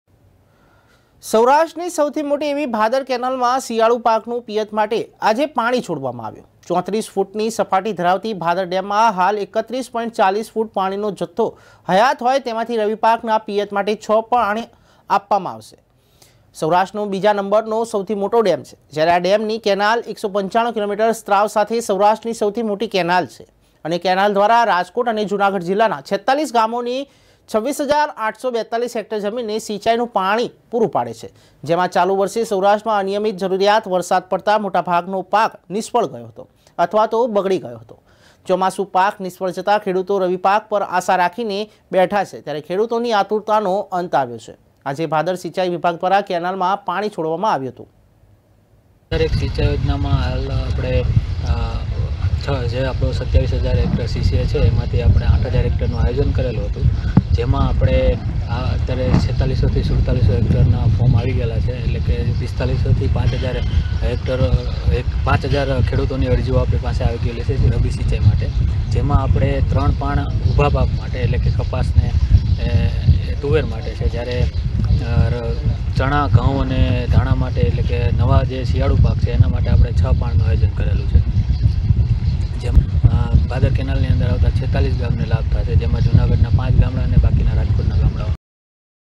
સૌરાષ્ટ્ર : ભાદર કેનાલમાં શિયાળું પાક માટે પાણી છોડવામાં આવ્યું
Top Headlines
News Flash
Also check out ✅ our official Divya Bharat News site: